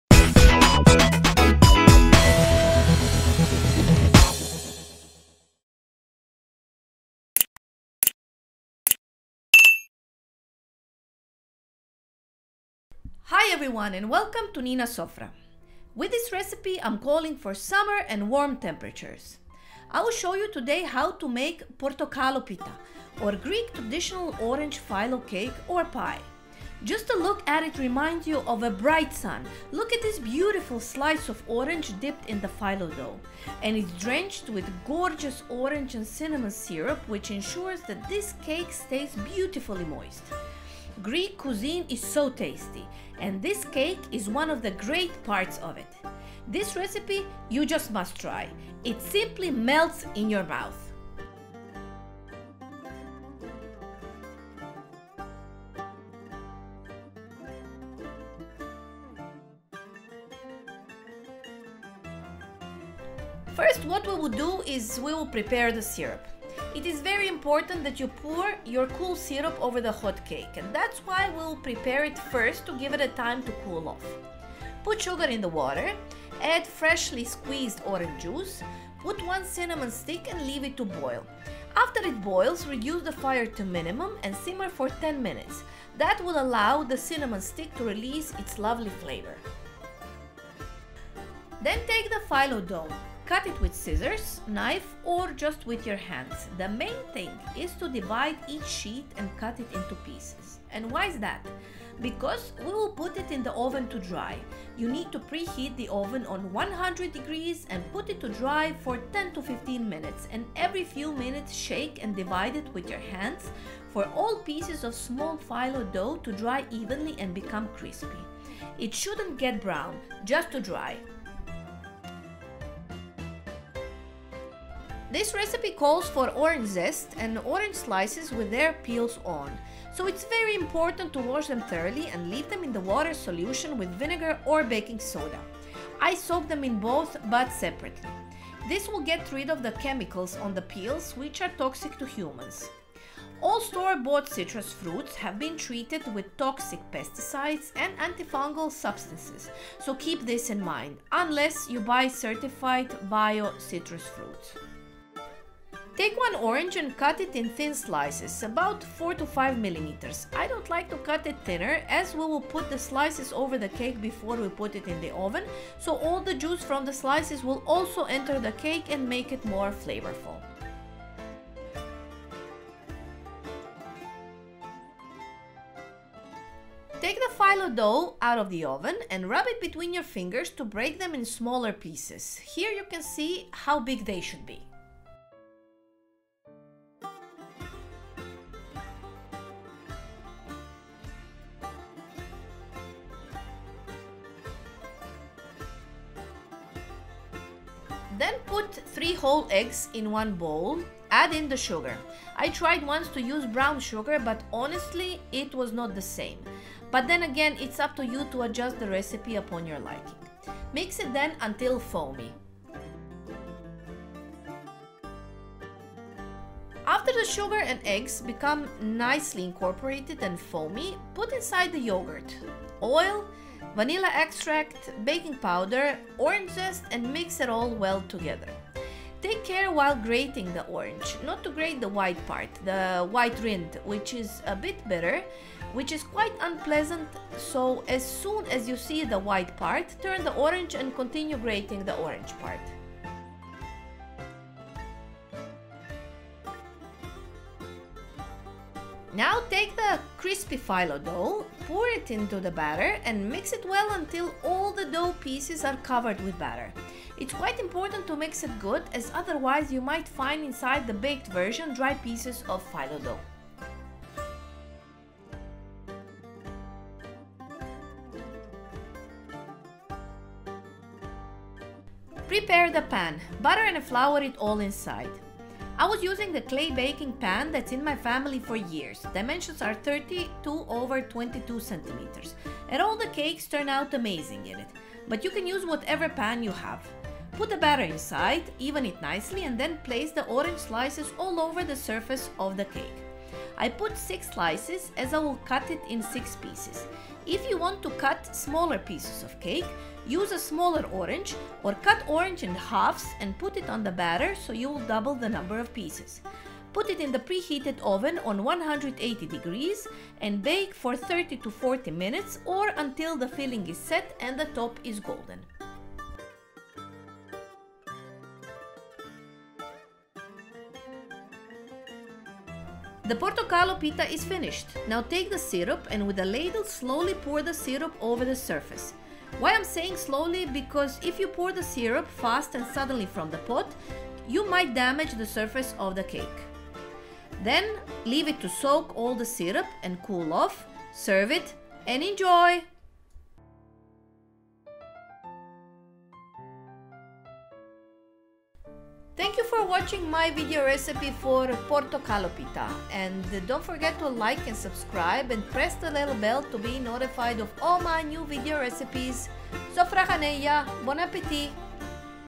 PORTOKALOPITA - Greek juicy orange phyllo cake / Grčki sočni kolač od naranče / ΠΟΡΤΟΚΑΛΟΠΙΤΑ!
BATTER / TIJESTO
 - 250 g sheets phyllo pastry / 250 g tijesta za savijače
 - 3 eggs / 3 jaja
 - 150 g sugar / 150 g šećera
 - zest of 2 oranges / kora od dvije naranče
 - 200 ml yoghurt / 200 ml jogurta
 - 200 g sunflower oil
 - 1 tsp vanilla extract / 1 čajna žličica vannili ekstrakta
 - 1 & 1/2 tsp baking powder /1 i 1/2 čajne žličice praška za pecivo

SYRUP / SIRUP
 - 200 g sugar / 200 g šećera
 - 300 ml water / 300 ml vode
 - juice of 2 oranges / sok od dvije naranče
 - 1 cinnamon stick / 1 štapić cimeta

1 orange cut in slices  / 1 naranča narezana na kolutove

Photo by Rene Lehmkuhl on Unsplash